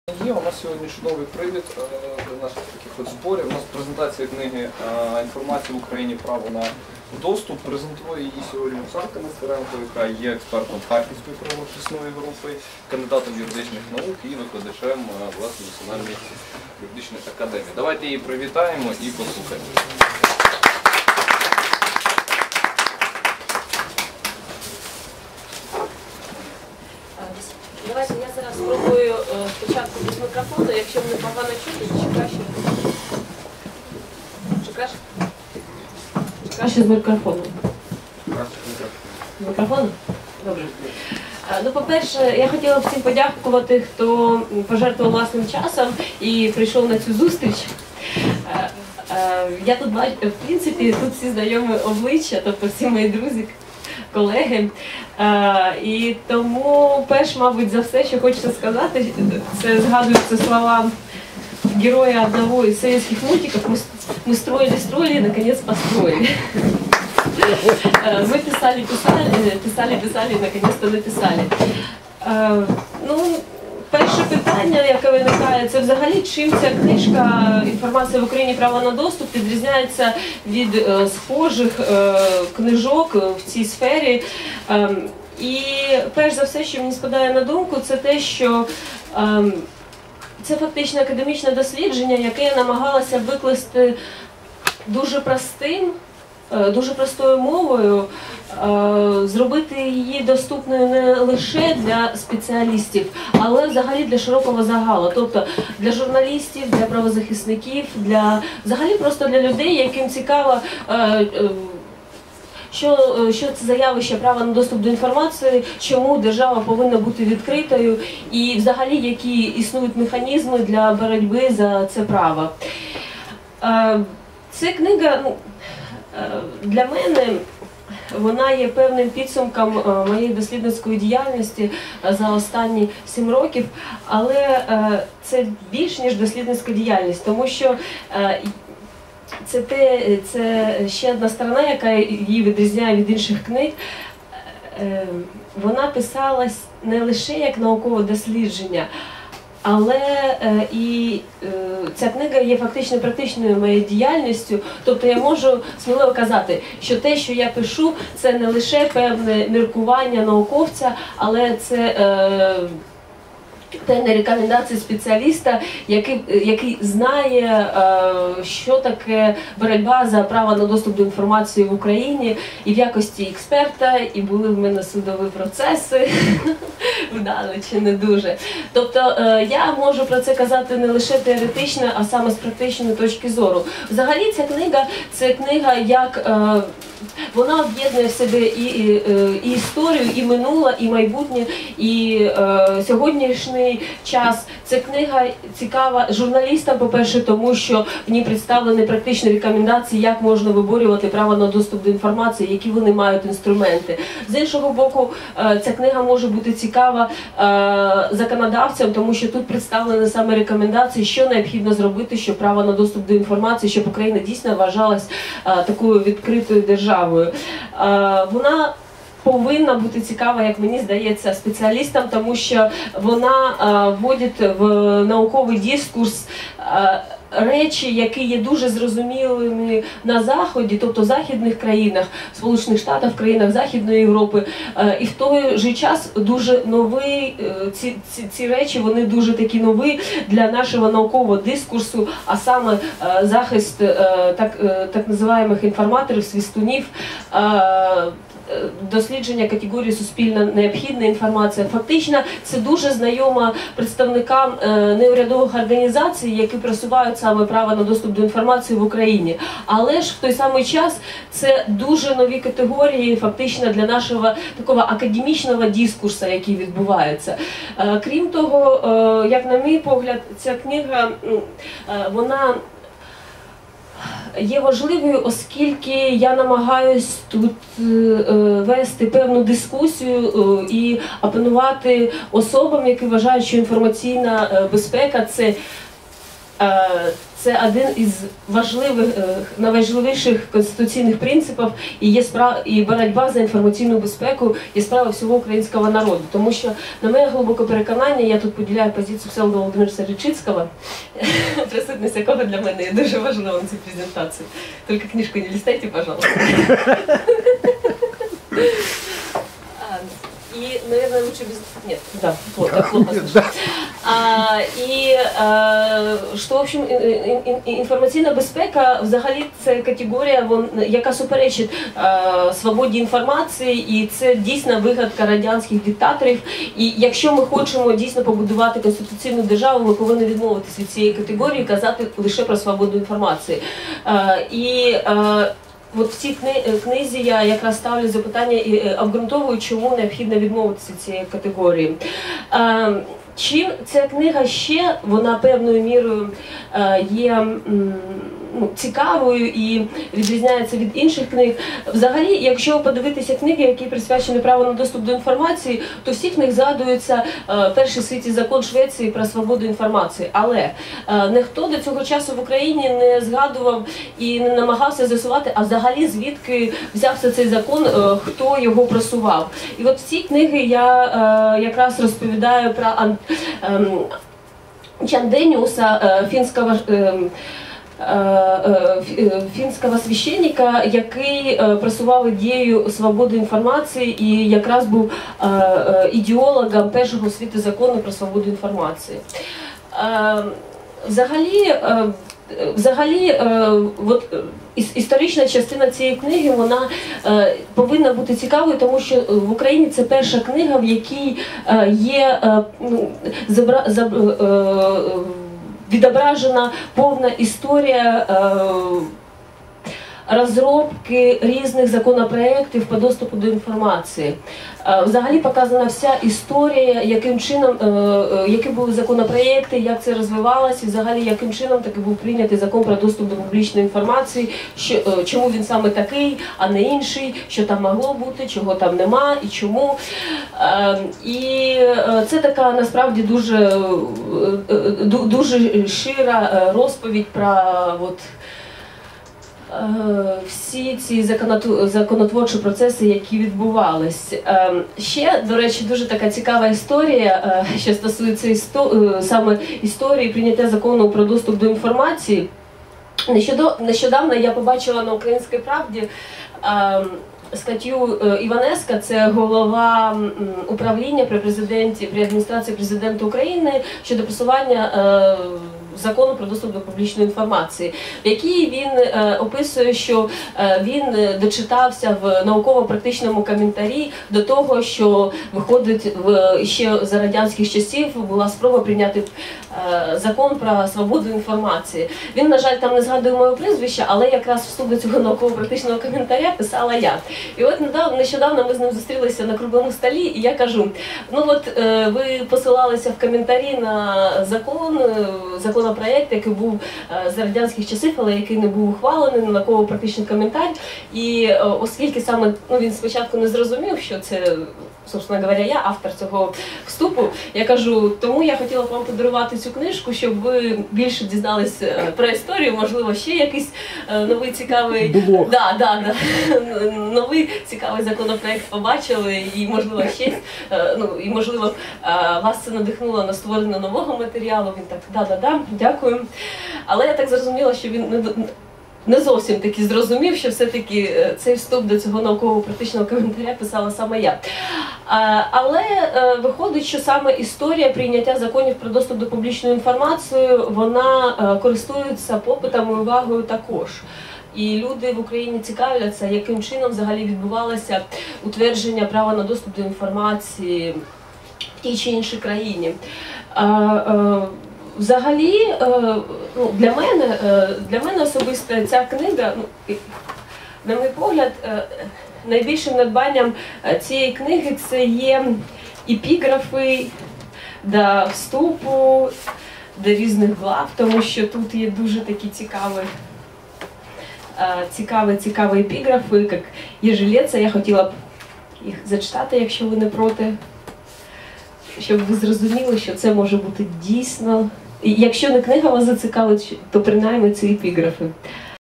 Інформація в Україні: право на доступ
Презентація нової книги Оксани Нестеренко. Вступне слово автора